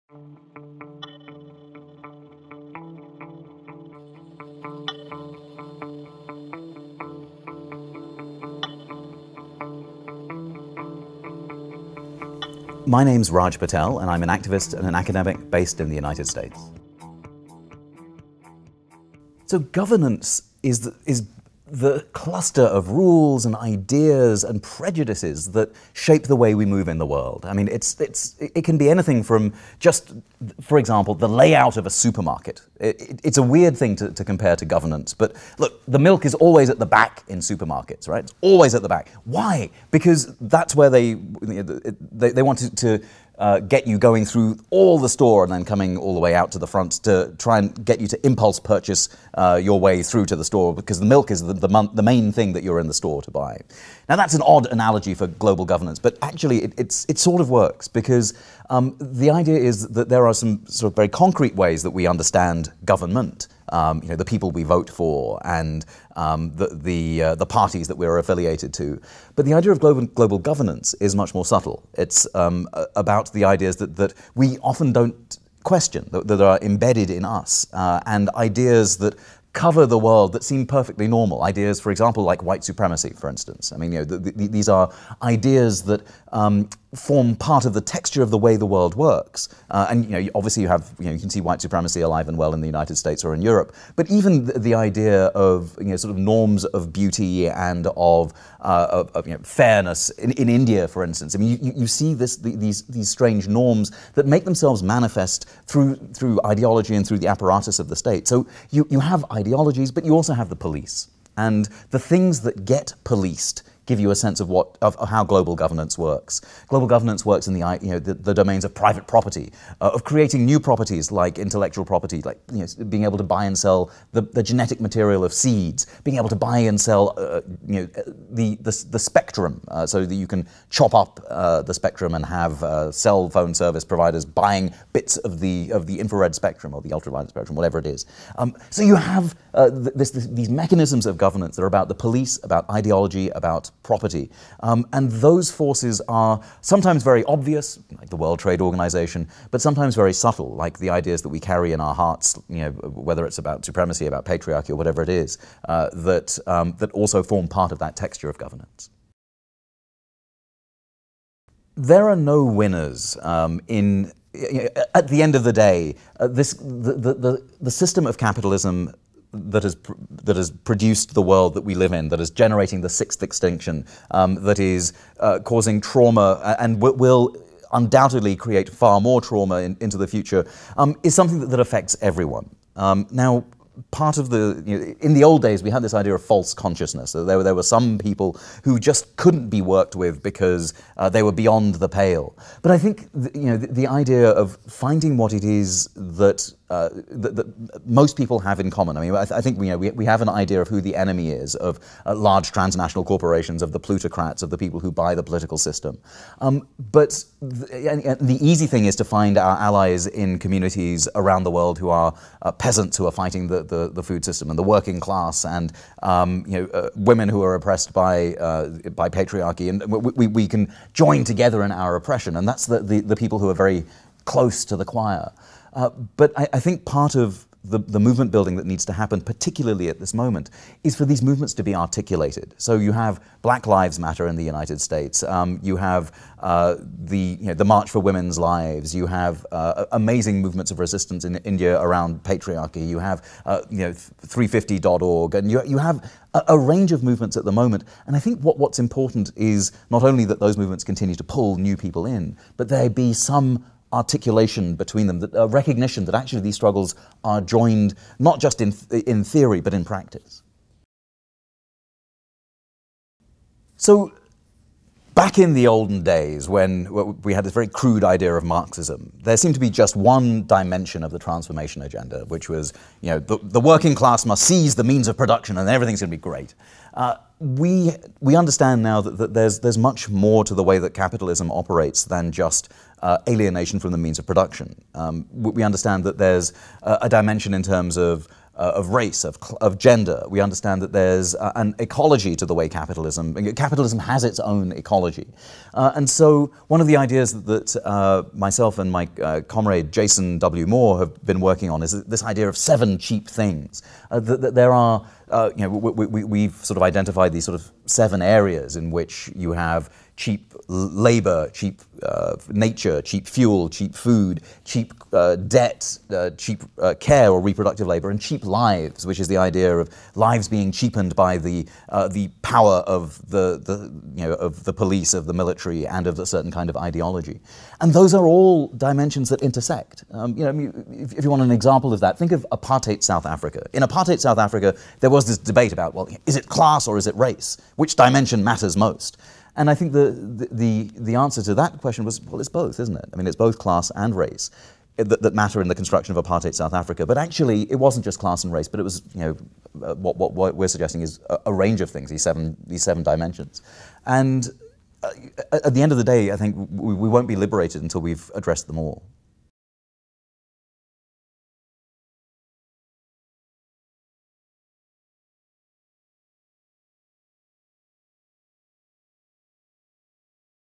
What is global governance? An interview with Raj Patel
Interview with Raj Patel at the Colloquium on Global Governance, Climate Justice & Agrarian Justice in The Hague.